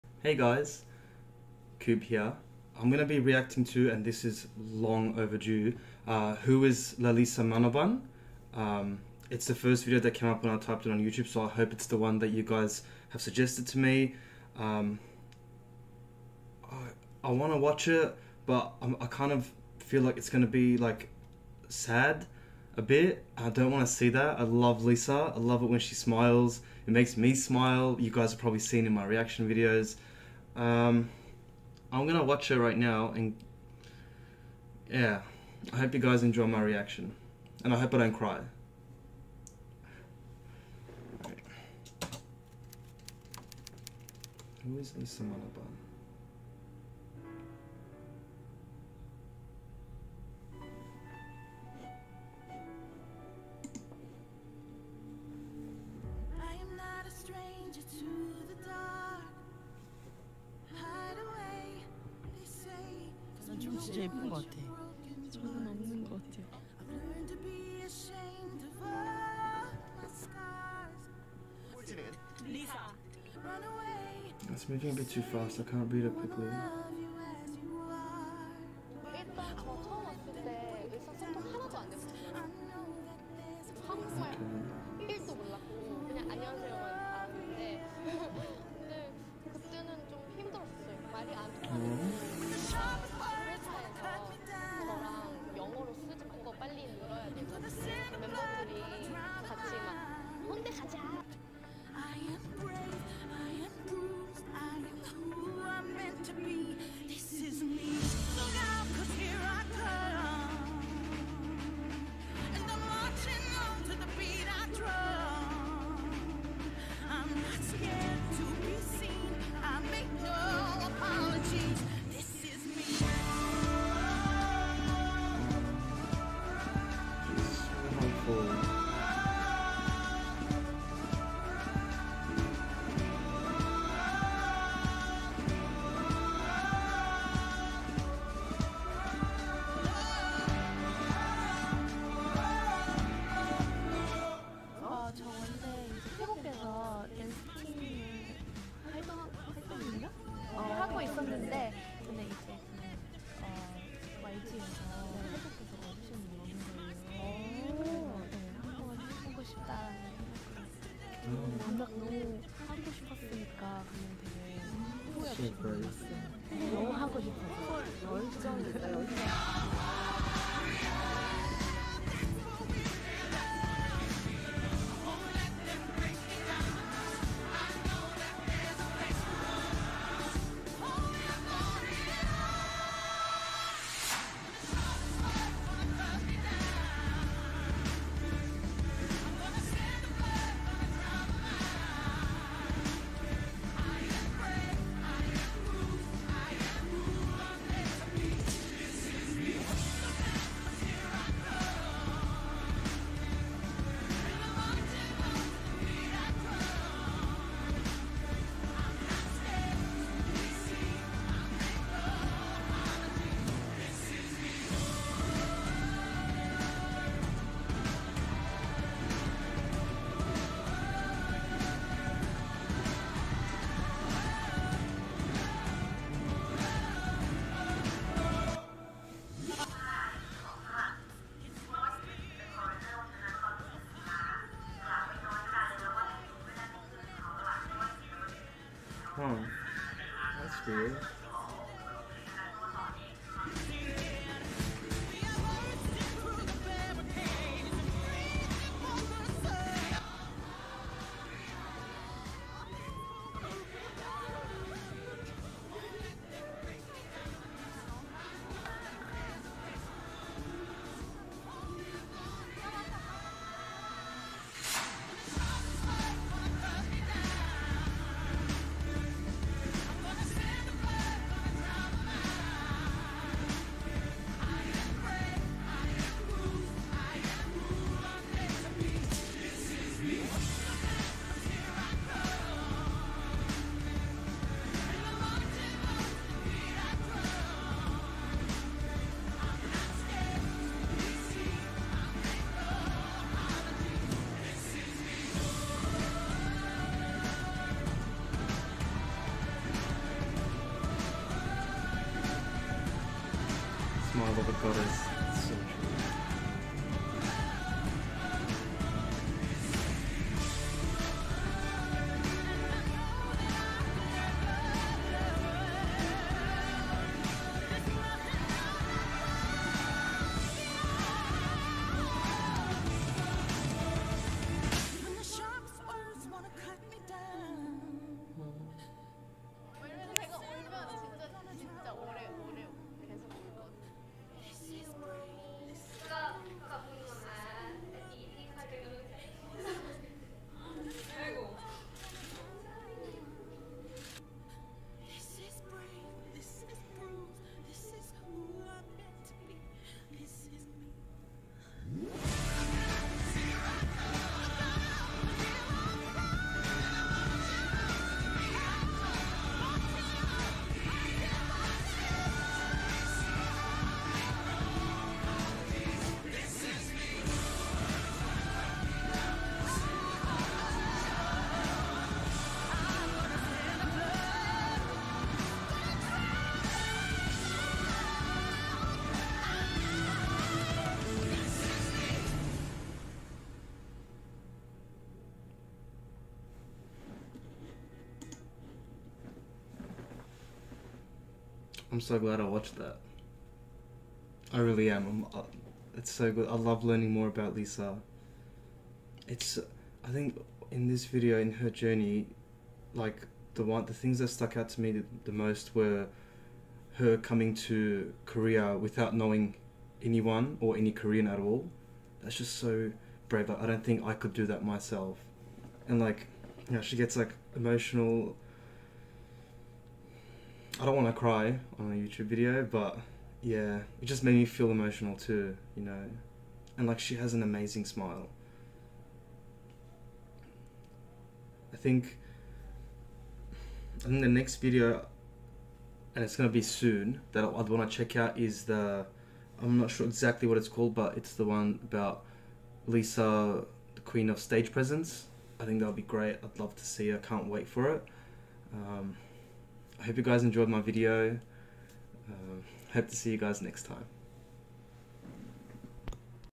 Who is Lalisa Manoban Blackpink Lisa's Journey [REACTION]
Some thing I have wanted to watch for a while, hope you guys enjoy my reaction!!